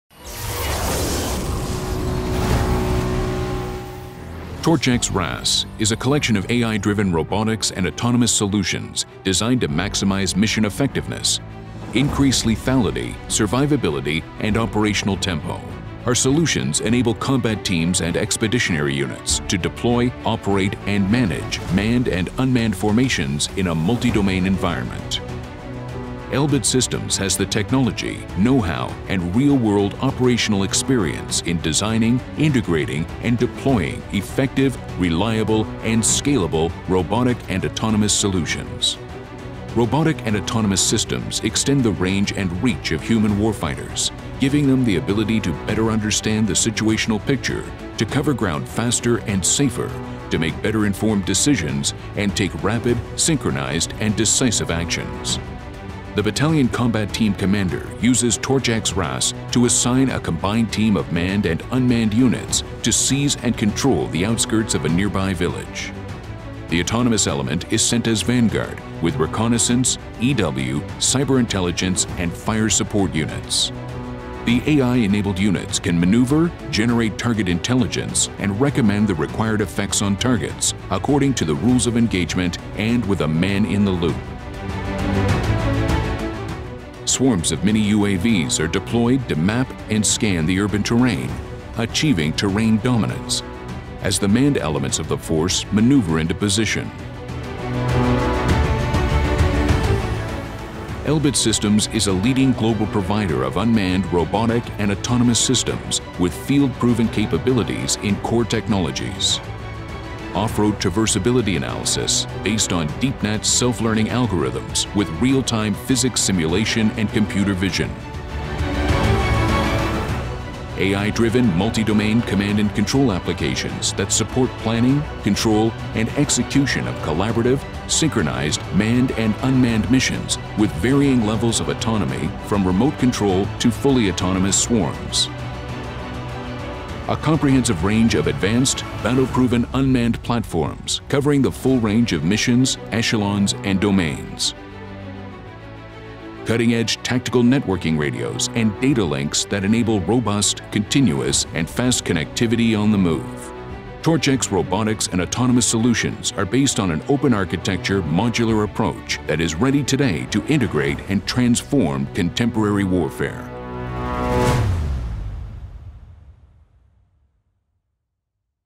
TORCH-X RAS
TORCH-X RAS (Robotics and Autonomous System) is a collection of AI-driven, robotics and autonomous solutions designed to maximise mission effectiveness and increase survivability, lethality and operational tempo. It enable combat teams and expeditionary units to deploy, operate and manage manned and unmanned formations in a multi-domain environment.

Robotic and autonomous systems extend the range and reach of human warfighters, giving them the ability to:

a. Better understanding the situational picture;
b. Cover ground faster and safer;
c. Maker better informed decisions; and
d. take rapid, synchronised and decisive actions.

TORCH-X RAS combines fast software-defined radio (SDR) networks with Robotic and Autonomous Systems controlled and monitored by leading edge Battle Management Systems (BMS). Leveraging our customers current investments, this provides the operational advantage warfighters need to fight and win in Joint Land Combat.

Elbit Systems of Australia understands how to protect, empower and support tomorrow's warfighter through leveraging BMS to support planning, control and execution of collaborative, synchronised, manned and unmanned missions.